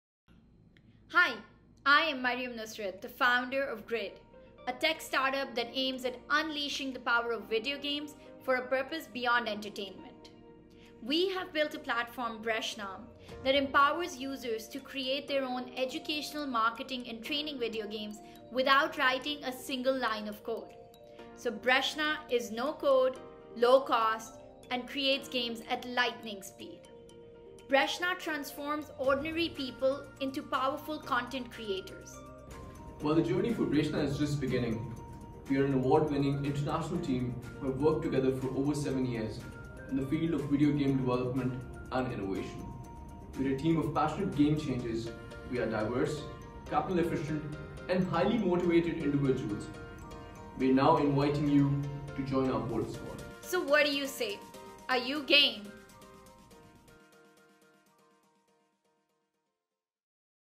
GRID feature as a Top 21 Startup of '21
Startup Name: GRID  - Gaming Revolution for International Development Inc.
What is the most exciting thing about you startup to date?
- Breshna operates at the intersection of 3 exploding tech trends, No-code, video games and NFTs;
- Breshna is a no code platform that empowers users to create purposeful NFT video games without writing a single line of code.
-Breshna changes how video games are created and used:
   - On the “create” side, we are leveraging the no-code movement to empower a non-tech audience to create their own video games.
   - On the “use” side, we are taking video games beyond entertainment and deploying their power for education, marketing, training, and story telling!

What is the company's unfair advantage?: Breshna's Bolt Squad!

Breshna is being built in public, which means we were building a community event before we started building our product!

Customer-built growth is triggered by the evangelism flywheel; When end users are empowered to build, they also become the 1st (& most authentic) sales people for our product. They’re proud of what the Breshna games they build & want to show it off.

Factors for growth in '21:
1) Familiarity with the pain point: Being a non-tech person who believes in the power of video games, I have always craved a tool that can empower me to make my own purposeful video games without engaging a gaming studio. Breshna democratizes content creation through video games.

2) Team: We are a team of 18 game changers who built Breshna's MVP and Alpha versions in less than 8 months. Our Alpha was #2 product of the day on Product Hunt, we have 1000+ registered users who have created 3000+ video games.

Connect with all of the Top 21 startups as well as our Startup Oasis community members